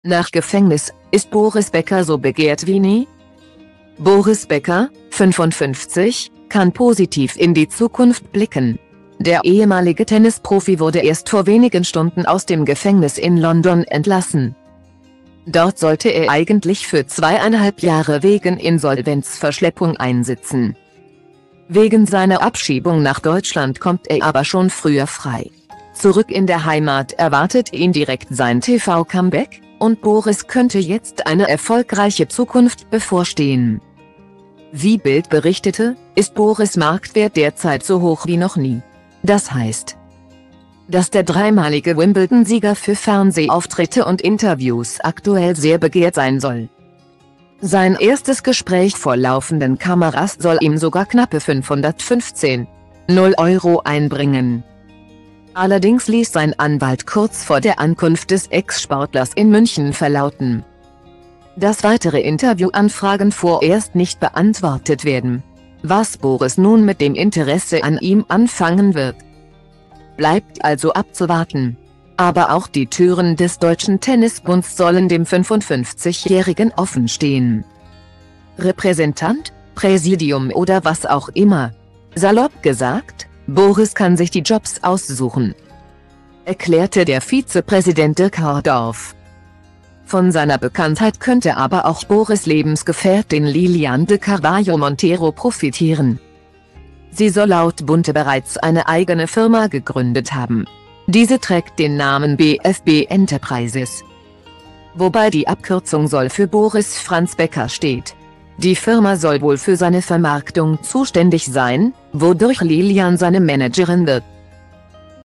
Nach Gefängnis: Ist Boris Becker so begehrt wie nie?
Boris Becker (55) kann positiv in die Zukunft blicken. Der ehemalige Tennisprofi wurde erst vor wenigen Stunden aus dem Gefängnis in London entlassen.Dort sollte er eigentlich für zweieinhalb Jahre wegen Insolvenzverschleppung einsitzen.Wegen seiner Abschiebung nach Deutschland kommt er aber schon früher frei. Zurück in der Heimat erwartet ihn direkt sein TV-Comeback – und Boris könnte jetzt eine erfolgreiche Zukunft bevorstehen.